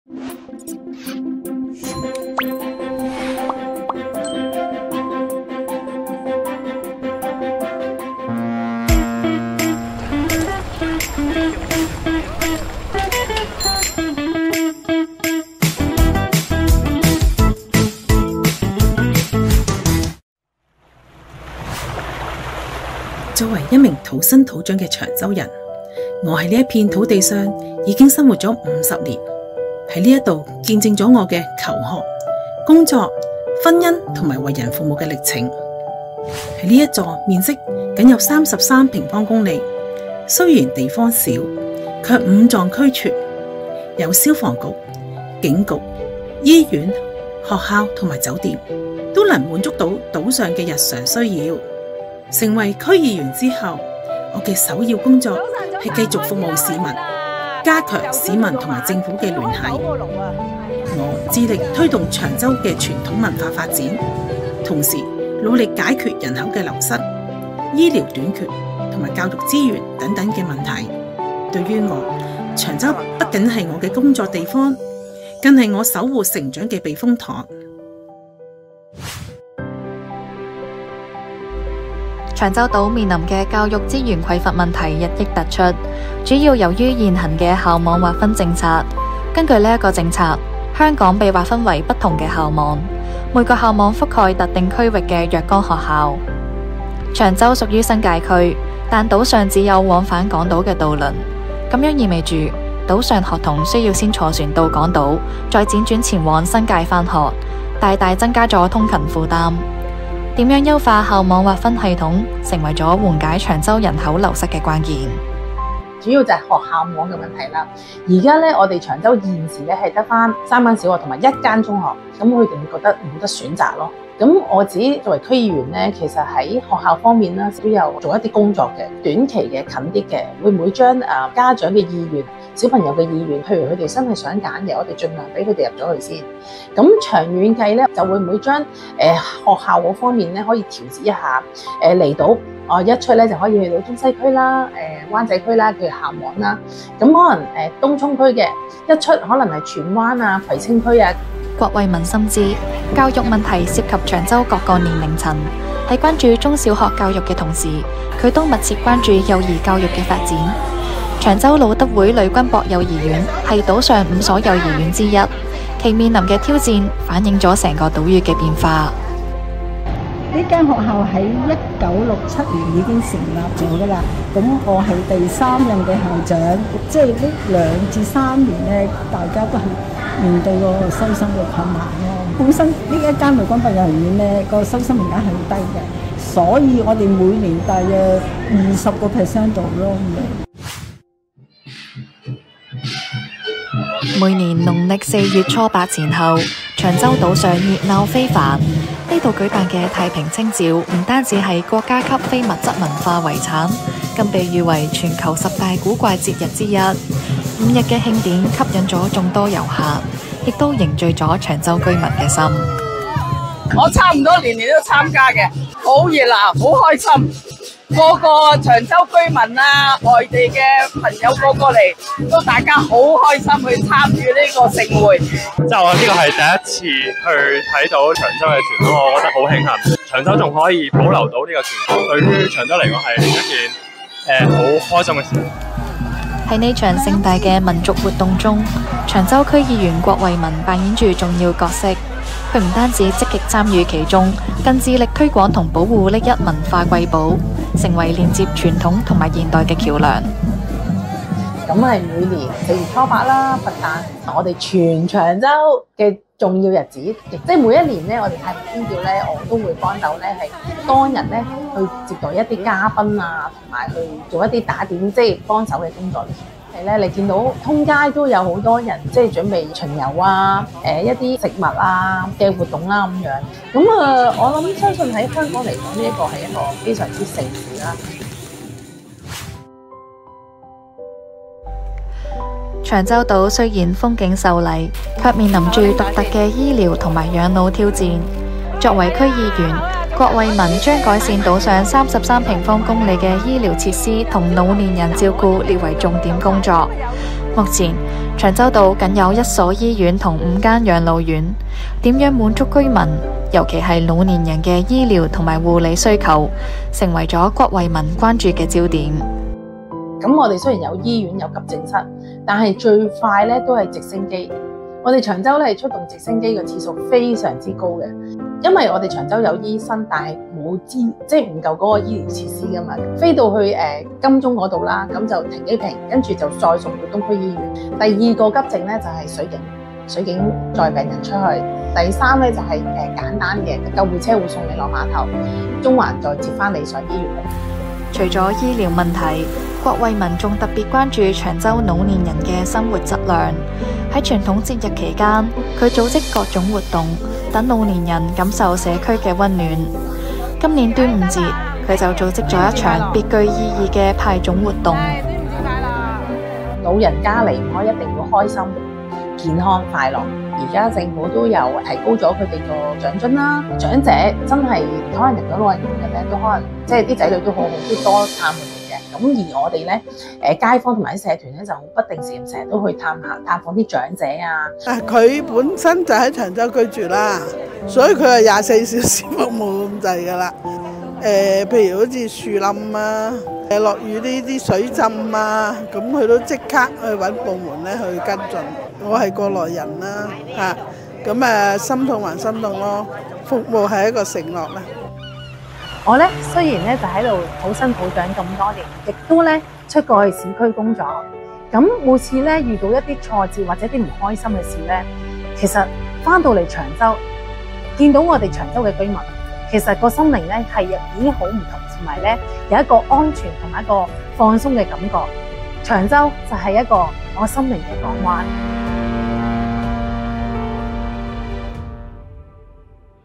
《區區小事》（七）離島區議員郭慧文：長洲是別人的旅遊打卡地 我的心靈港灣
提到香港長洲島，許多人腦海中浮現的可能是獨特民俗「太平清醮」 、「搶包山」 比賽，又或是「便靚正」 的海鮮大排檔。對於土生土長的長洲人、現任香港特區離島區議員郭慧文來說，這個小島承載的遠不止是旅遊景點那麼簡單，生活了數十年的長洲島見證了她的求學、工作、婚姻和為人父母的歷程，如今更是她擔任區議員後承擔的一份責任。